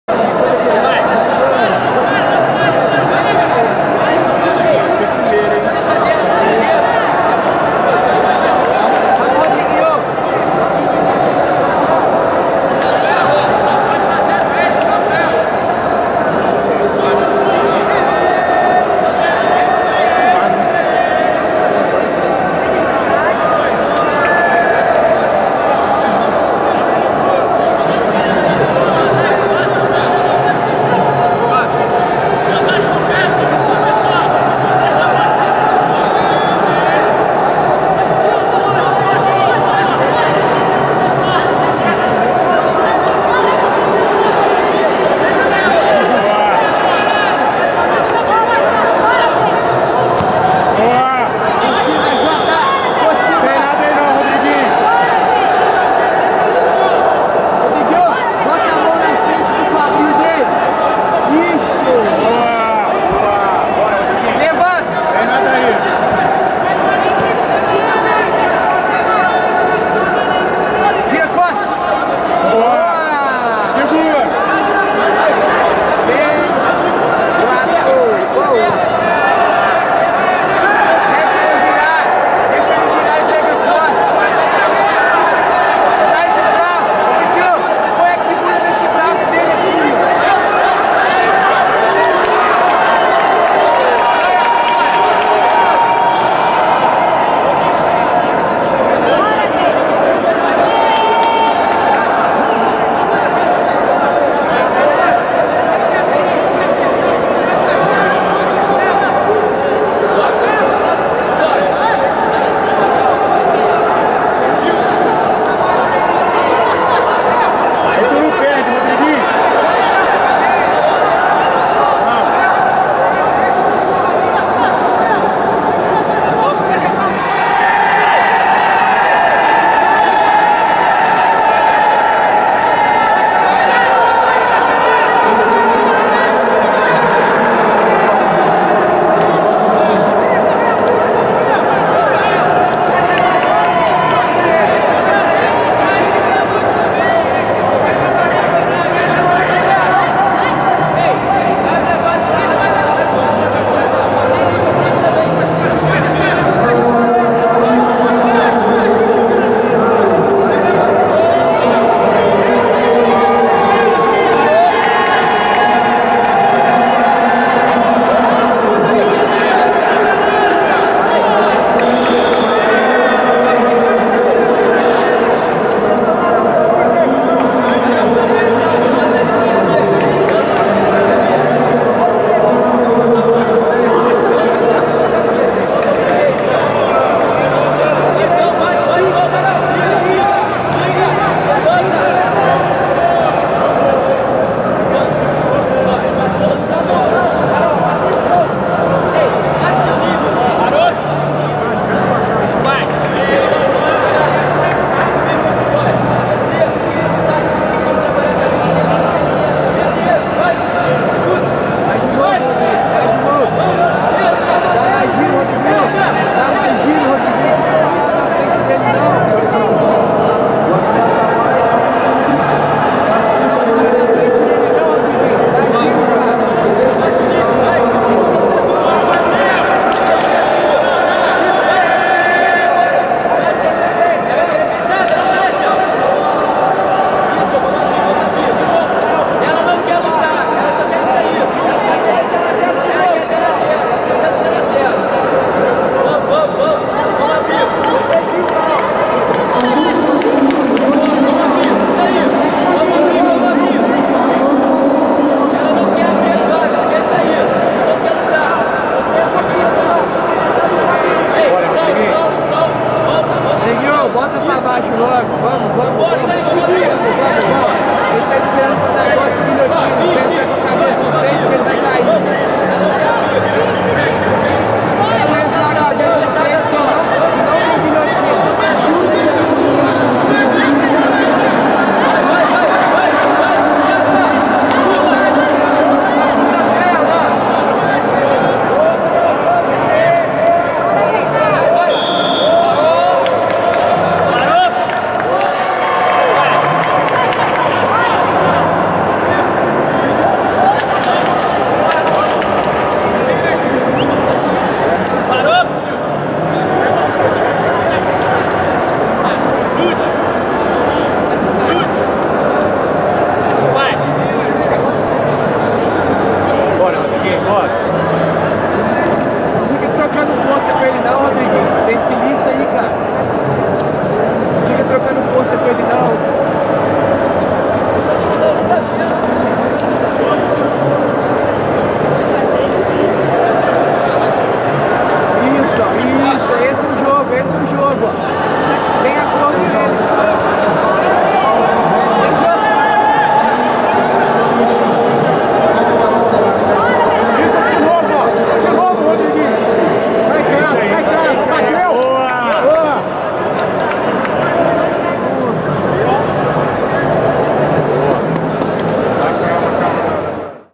Rodrigo Costa vs Bernardo Dib / Belo Horizonte Winter Open 2019
No-Gi/Black-Belt/Adult/Male/Light - Final